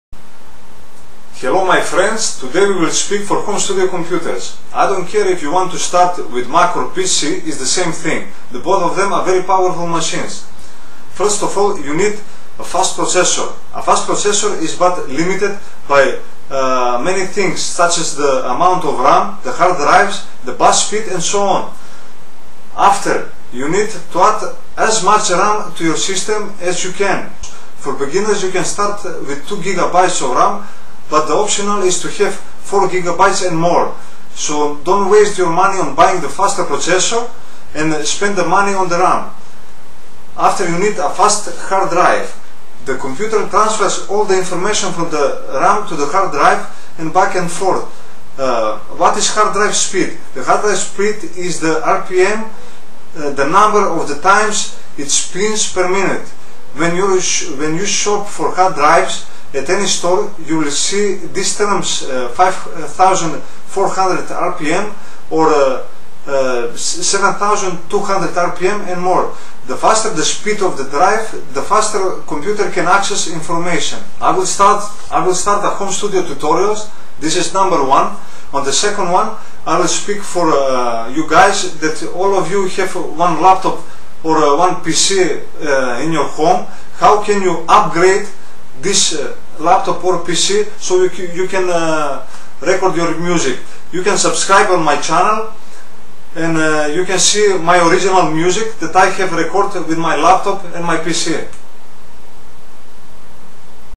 HOME STUDIO TUTORIALS #1 COMPUTER
In this video i will show you what computer hardware you need for Home Studio Recordings.I will start very simple home studio tutorials for better music recordings.In the second part i will show you how you can upgrade your laptop or pc hardware to be suitable for music recordings.